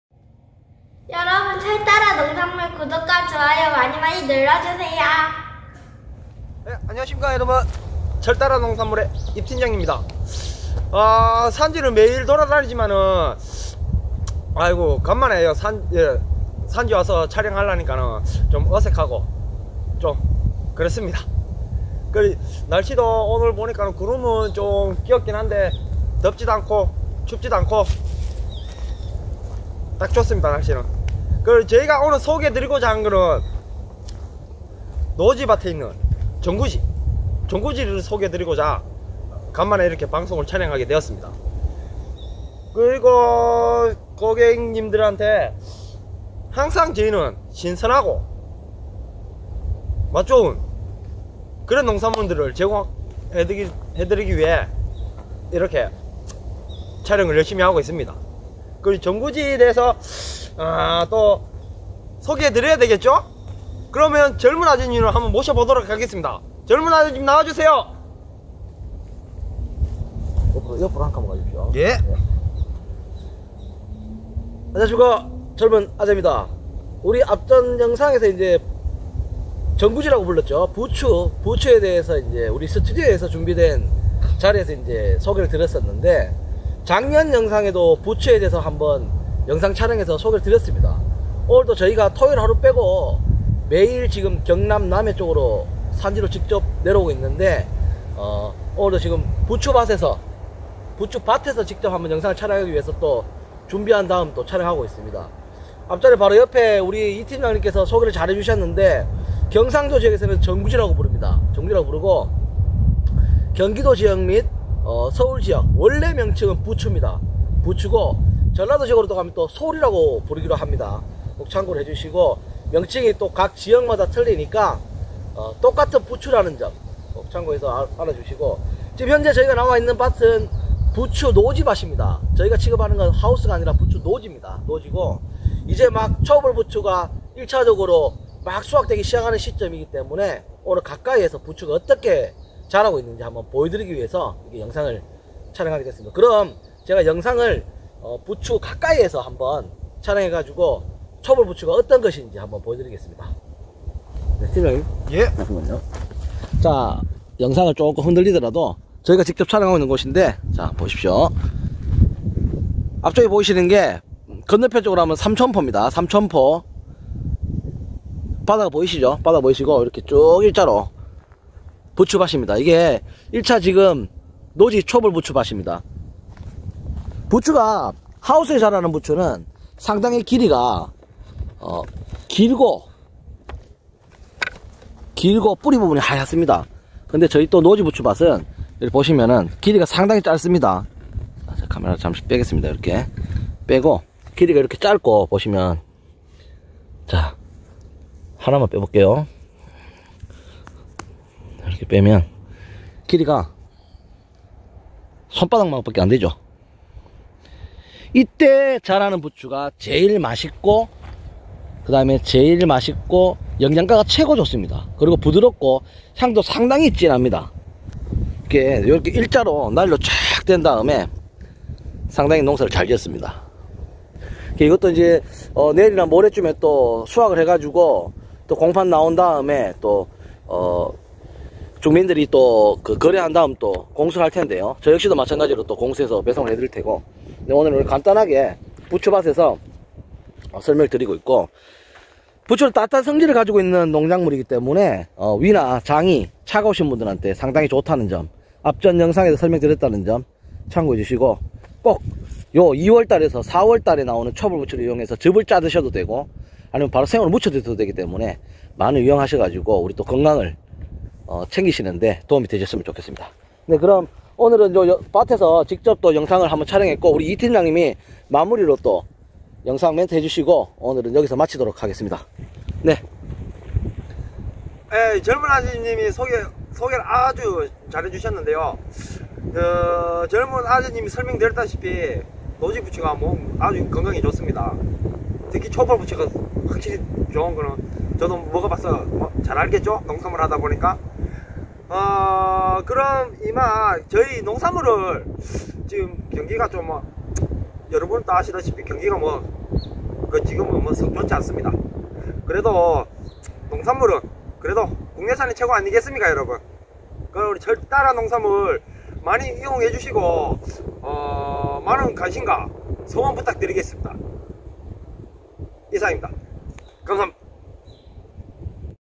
부추밭에 나왔어요~!!
정말 오랜만에 부추밭에 와서 농산물 촬영을 했습니다 .
매일 다니는 산지지만 농산물 실어야되지 옮겨야되지 등하다보니 시간이 없어 매일 산지영상을 올려드리지 못했네요^^

오늘은 경남 남해 초벌 부추밭에서 정구지(부추)에 대해 영상을 촬영해봤습니다~